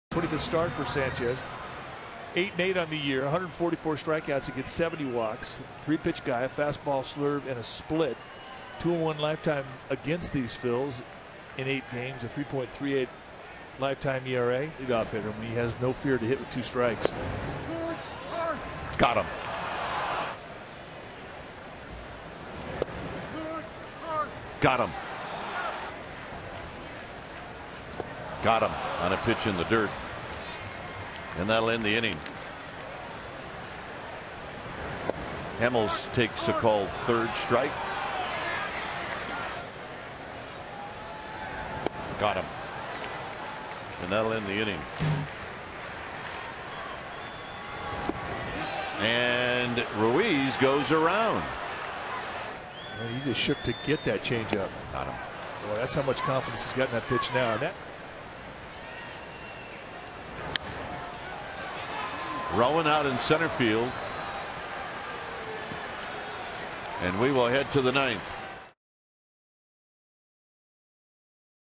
Sanchez's stellar start
SF@PHI: Sanchez hurls eight frames of one-run ball